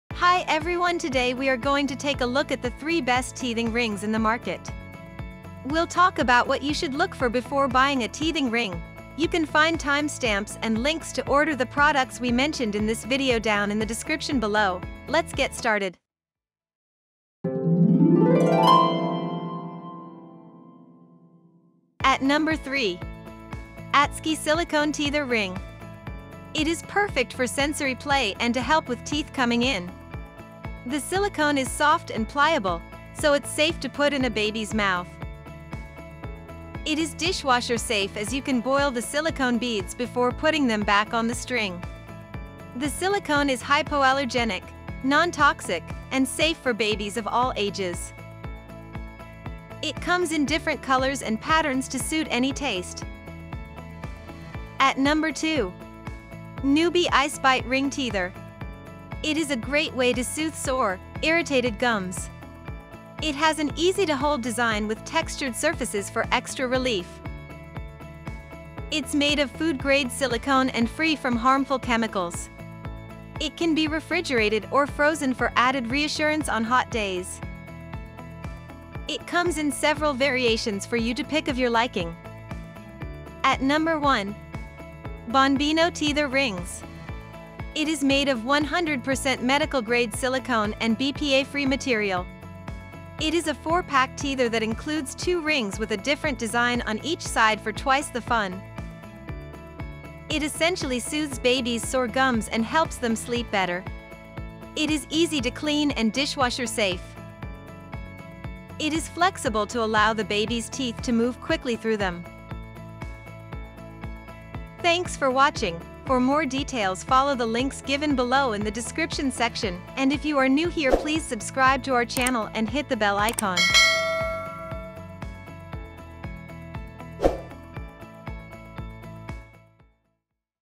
Best Teething Rings for Babies With Teething Pain
Follow the links given below to check the product prices:

 BONBINO TEETHER RINGS:

NUBY ICEBITE RING TEETHER:

ATSKY SILICONE TEETHER RING:

0:00​​  Best Teething Rings for Babies With Teething Pain
0:22  ATSKY SILICONE TEETHER RING
0:55  NUBY ICEBITE RING TEETHER
1:24  BONBINO TEETHER RINGS
1:55  Closing Remarks & Subscribe To Our Channel